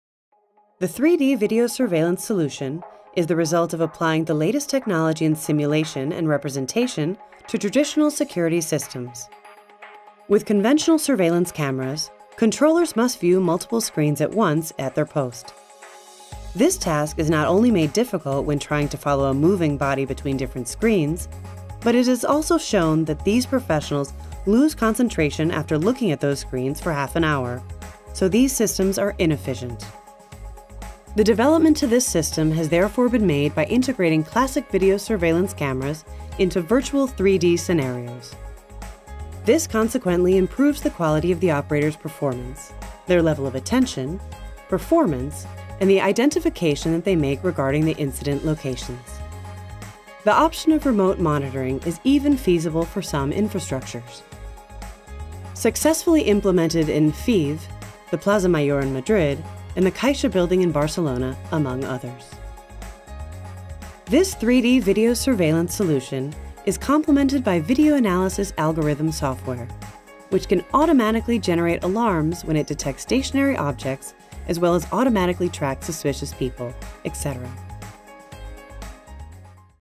Videovigilancia 3D - English
On a 3D virtual environment that replicates the infrastructure to be monitored , the actual images taken by the classic video surveillance cameras are integrated.